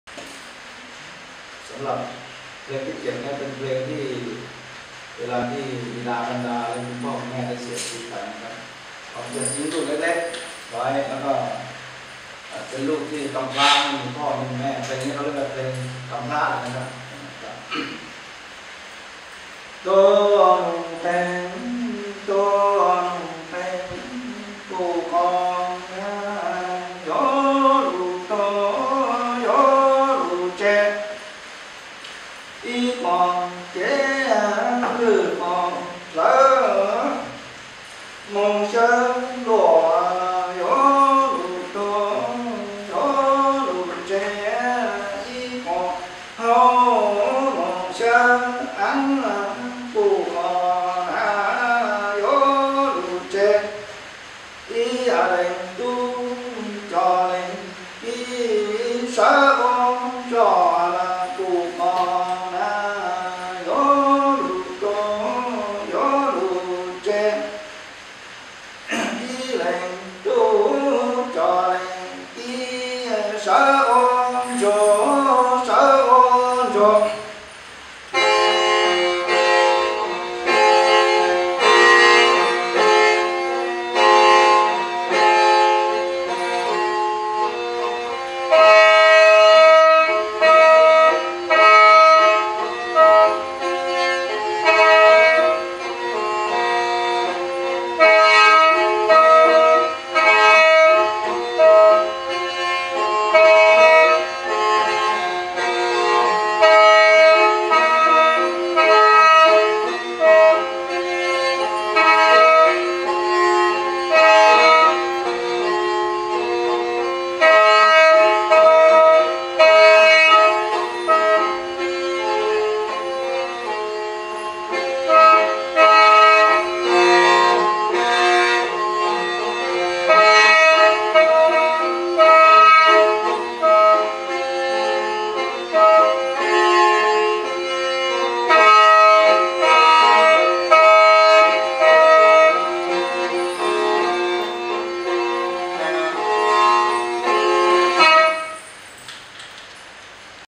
นพดล ศักดิ์เจริญชัยกุล แคนม้ง เพลงที่ 7
นพดล ศักดิ์เจริญชัยกุล ผู้ช่วย ผอ.ศูนย์พํมนาราษฎรบนพื้นที่สูง จ.พิษณุโลก  แคนม้ง  เพลง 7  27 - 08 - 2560 ถ่ายภาพ กี่ ช้างตว๋า  โครงการอนุรักษ์ฟื้นฟูการเป่าแคนม้ง  สนับสนุนโดย สสส และ สมาคม IMPECT